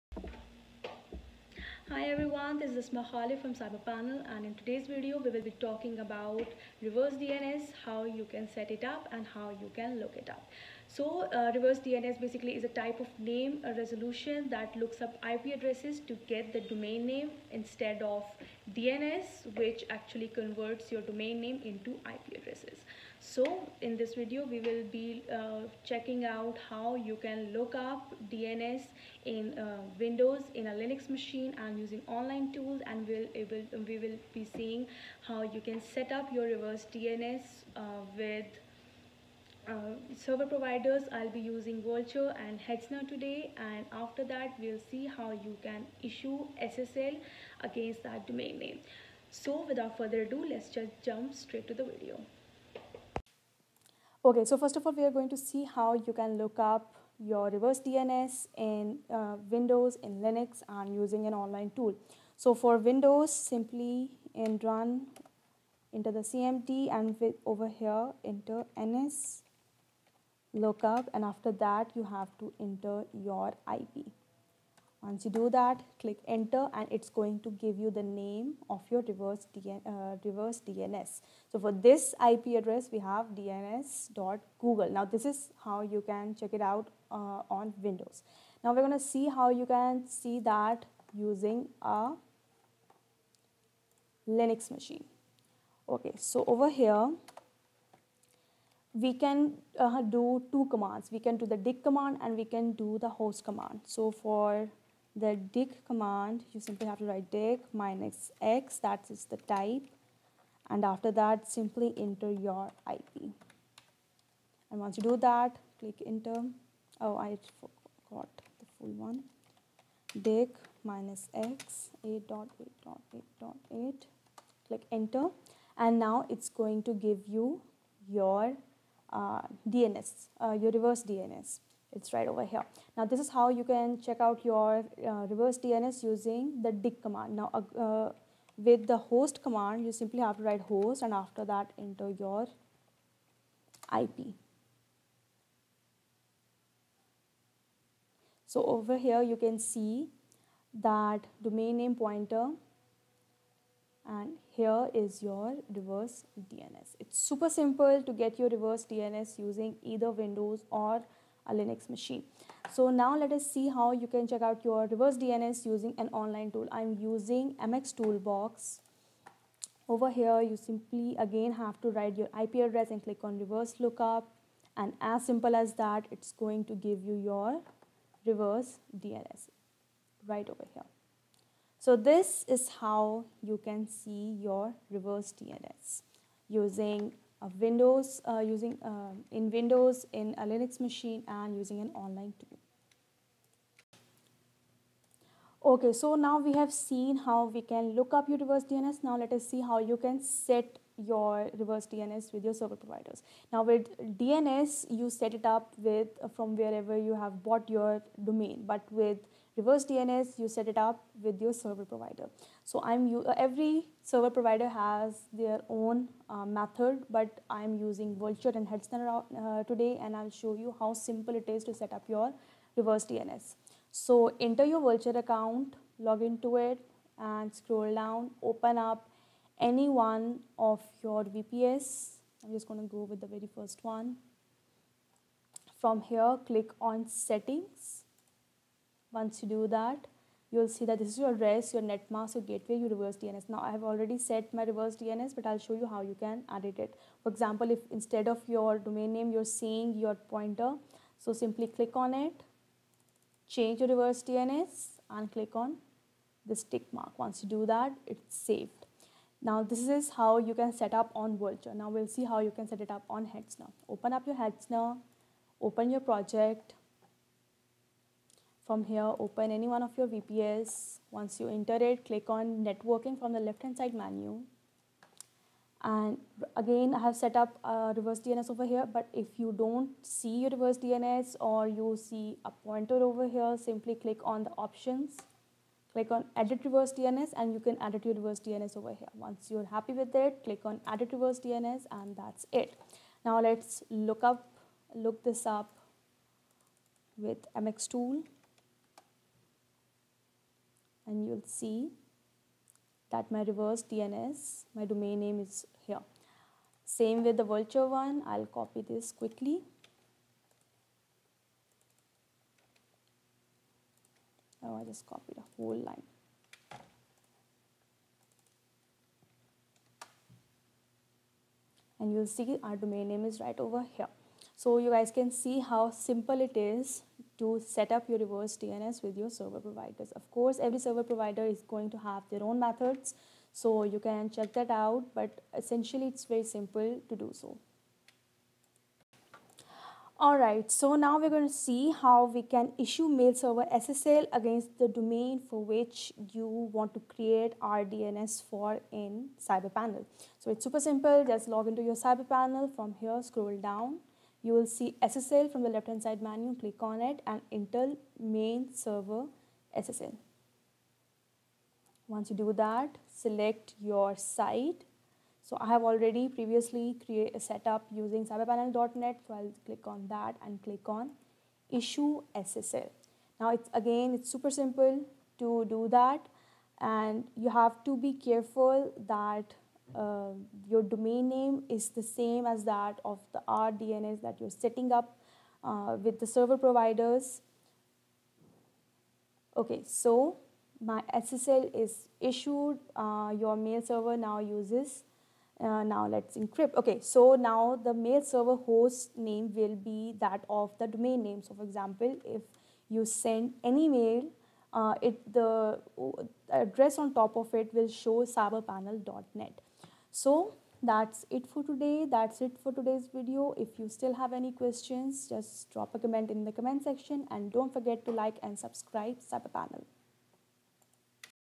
How To Perform Reverse DNS Look Up | What is rDNS? | How to set rDNS
In this video we will explain what is rDNS and how you can set it up with Vultr and Hetzner.